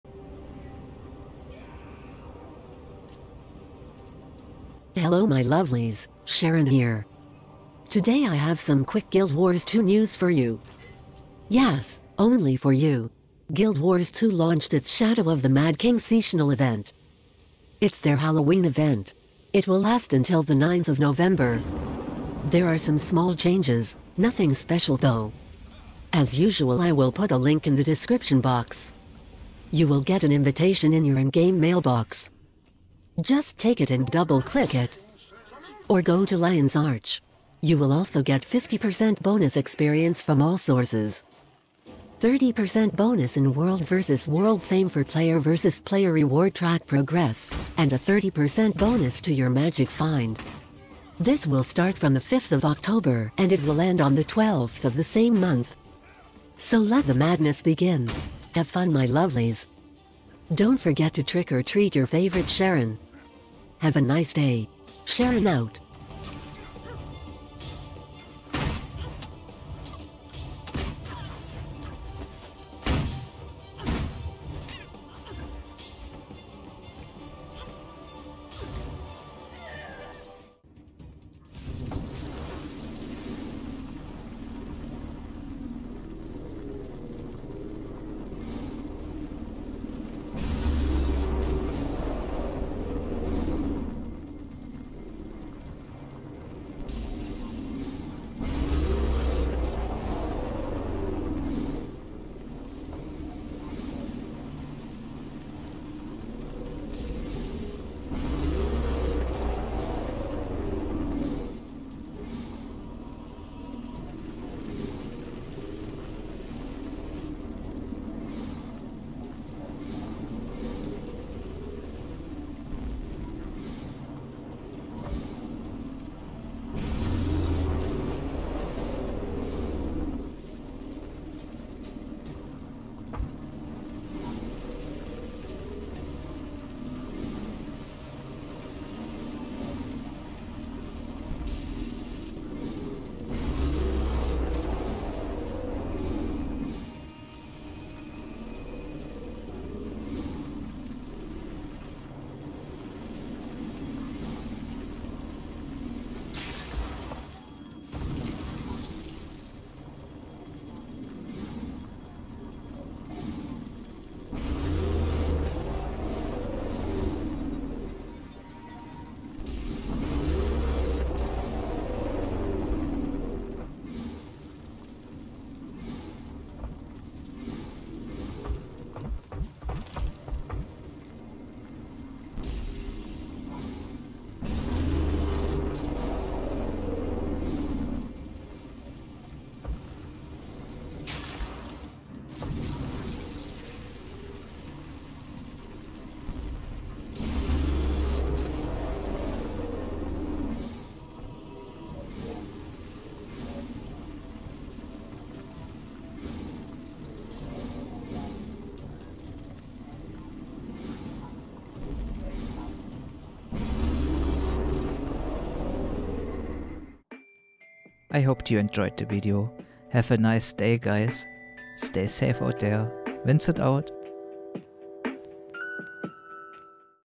Guild Wars 2 News with Sharon: Halloween Event (Shadow of the Mad King) 2021
The Shadow of the Mad King event started in Guild Wars 2! It's Guild Wars 2 version of Halloween! Get your Information right from Sharon herself! What is your favourite event in Guild Wars 2? Let us know! 😍